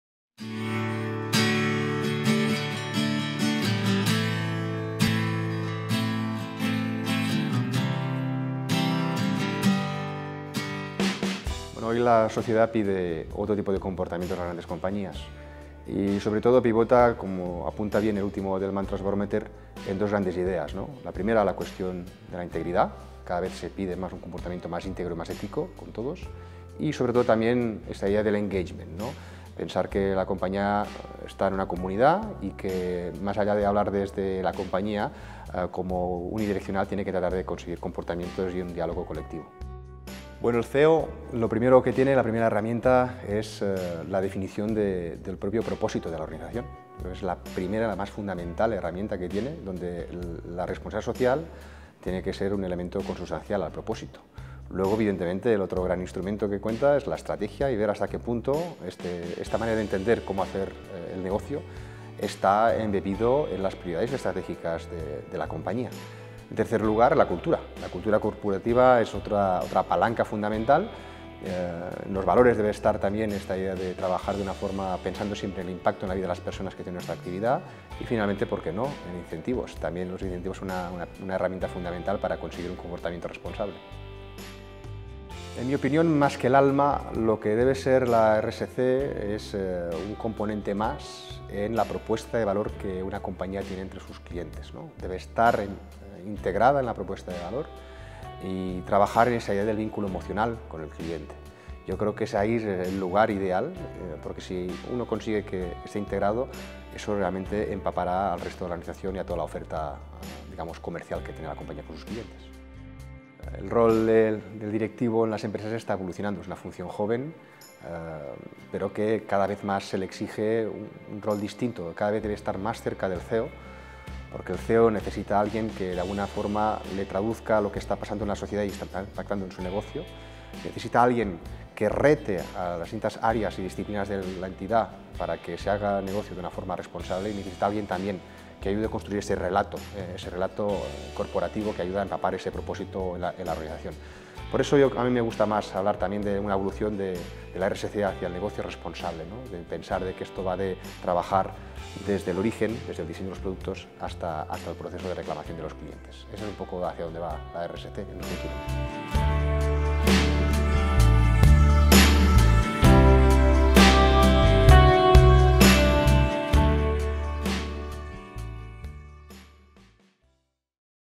Responsabilidad social empresarial: objetivos, estrategia, cultura e incentivos
Toni Ballabriga, presidente de DIRSE, nos habla sobre responsabilidad social empresarial (RSE), y ética empresarial.